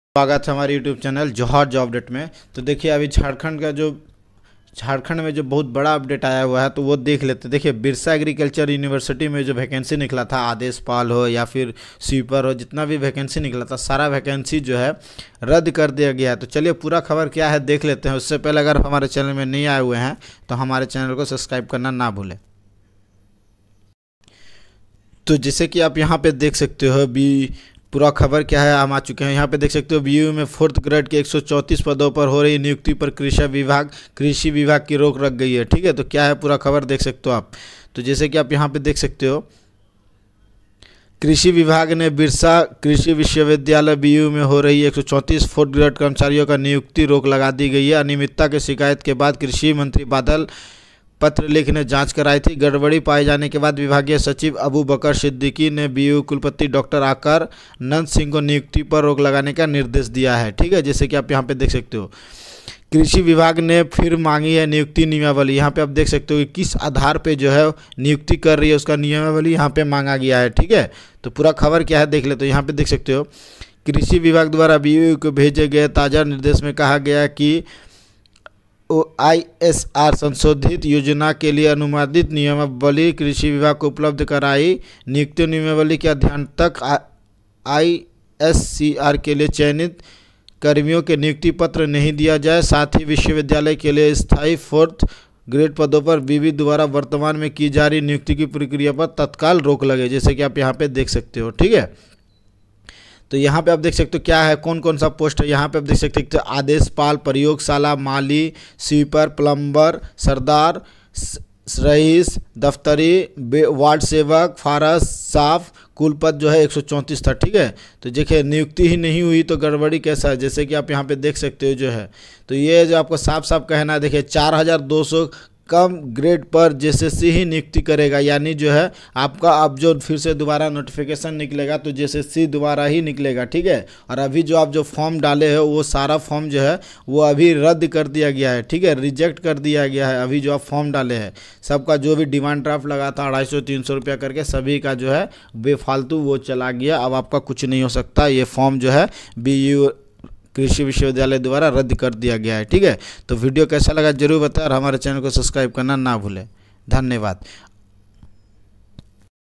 Birsa Agriculture University, Ranchi Vacancy 2023।।नया अपडेट सभी का फार्म रद्द कर दिया गया है
योग्यता - 10वीं  Post Name- पियून, माली, स्वीपर, दफ्तरी, सेवक, पलंबर, हेल्पर इत्यादि आवेदन की तिथि बढ़ाई गई है,